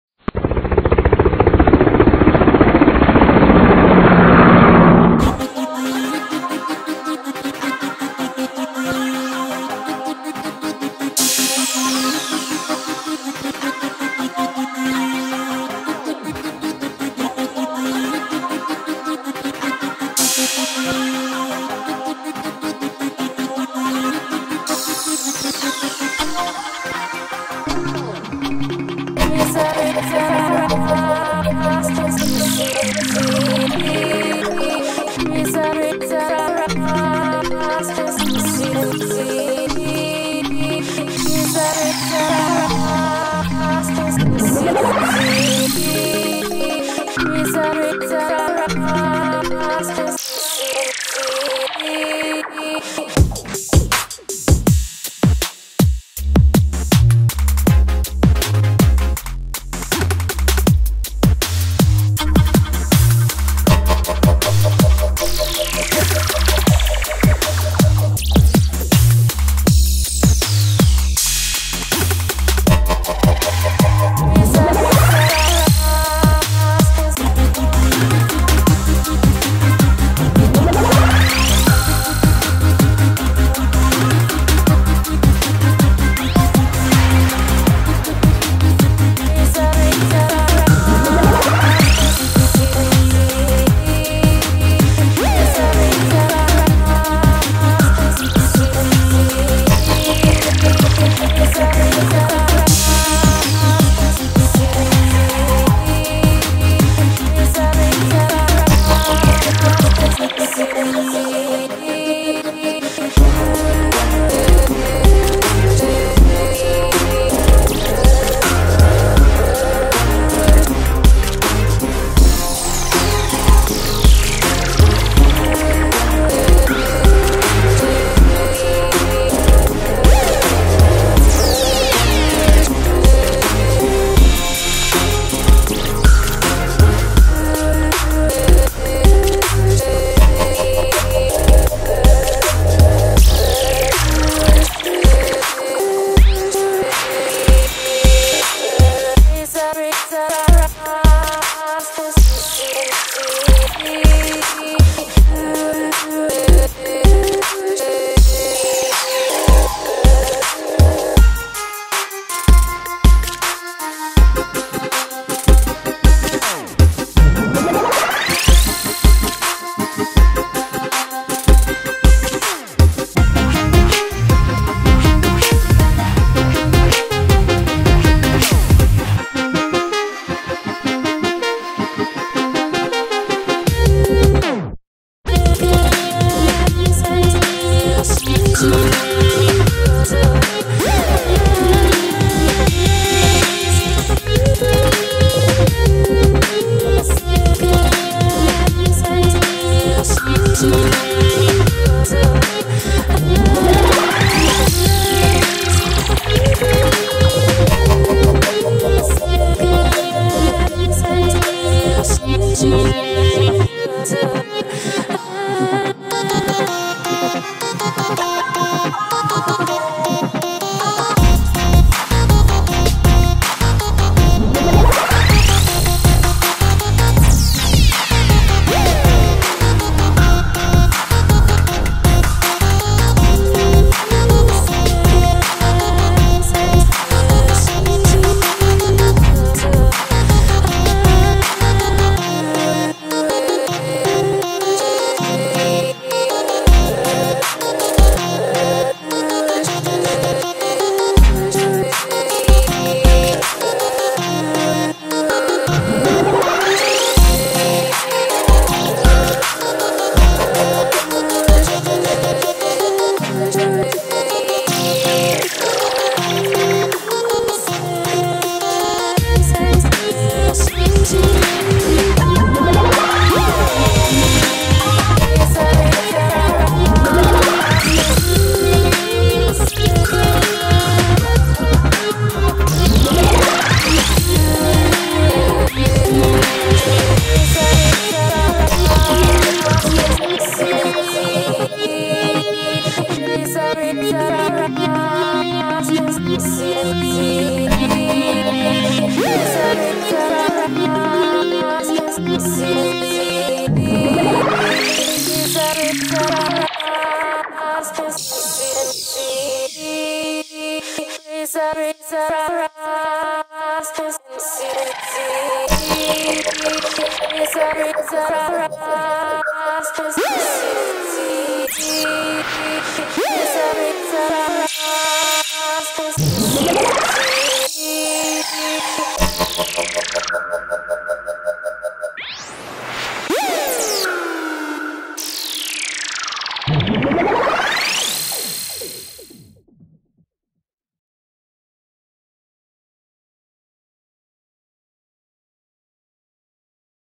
244groove/# sound wave surfing stunt / music-video original tracks.
This track is a video editing original arrangement by 244groove and a music video by original soundtrack.

The music production and arrangement editing are original tracks by 244groove.